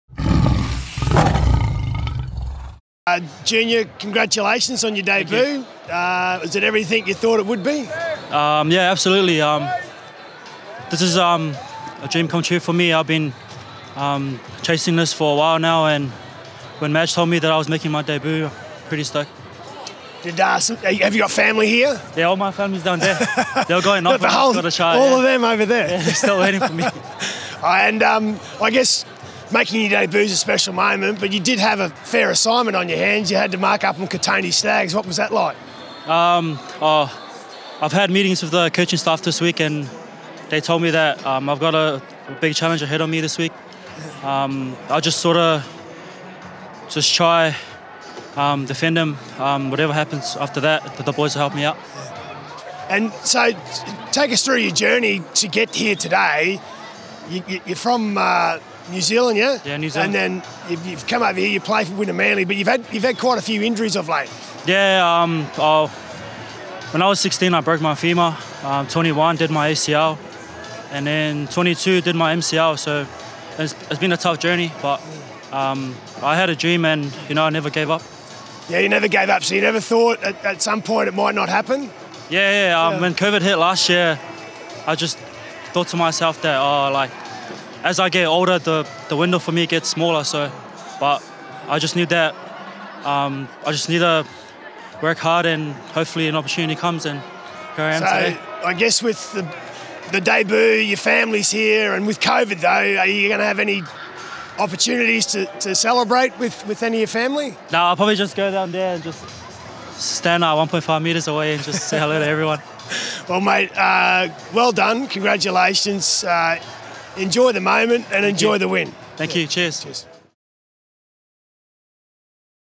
Junior Pauga reflects on NRL debut
Wests Tigers debutant Junior Pauga speaks about his journey to the big-time and what his next goal is now that he's played his first NRL game.